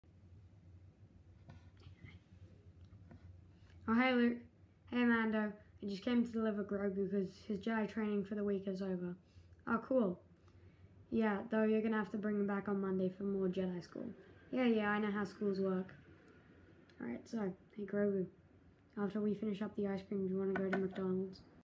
Grogu’s (baby yoda/the child) weekends be like.
Ik Jedi schools (if u can even call them that) probably don’t have weekends but it’s a funny idea